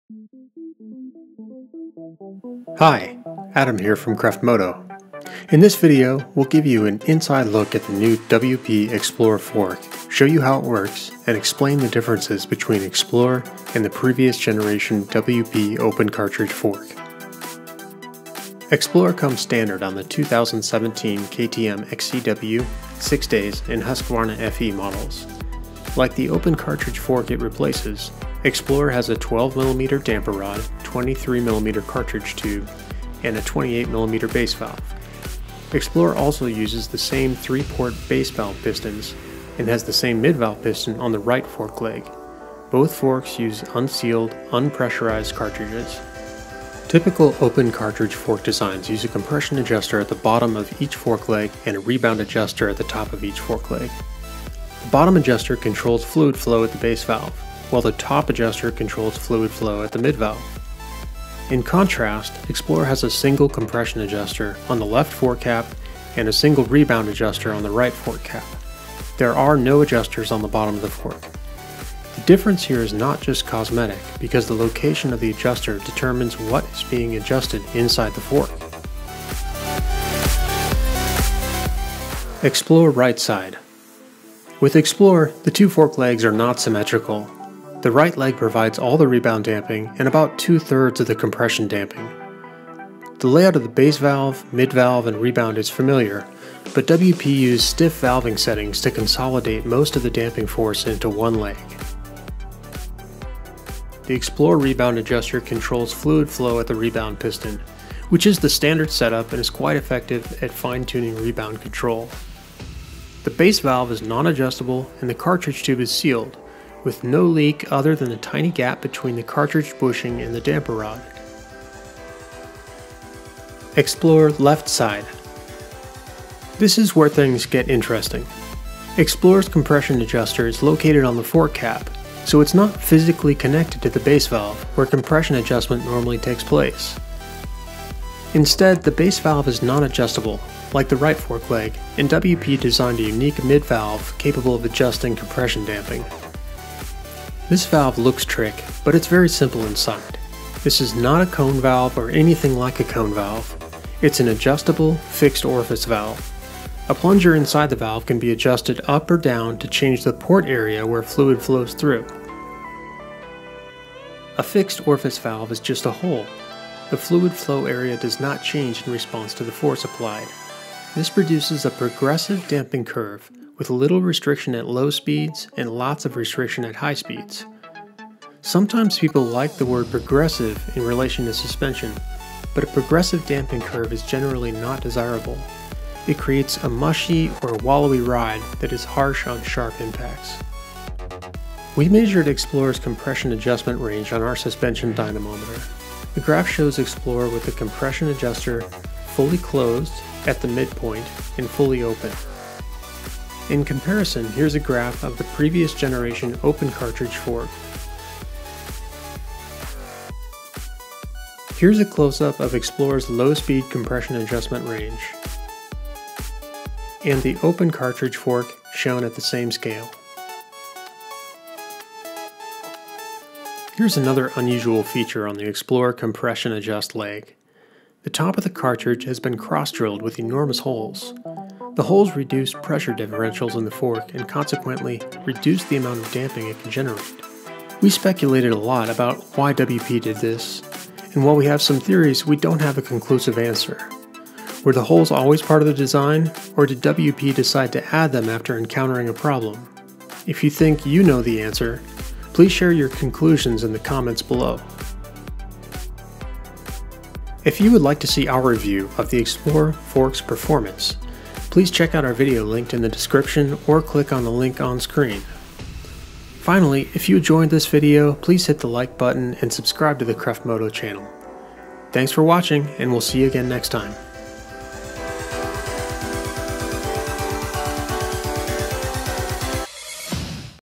INSIDE: WP Xplor 48 Fork
This video provides an inside look and technical explanation of the new WP Xplor fork that comes on 2017 KTM XC-W, KTM Six Days, and Husqvarna FE and TE models.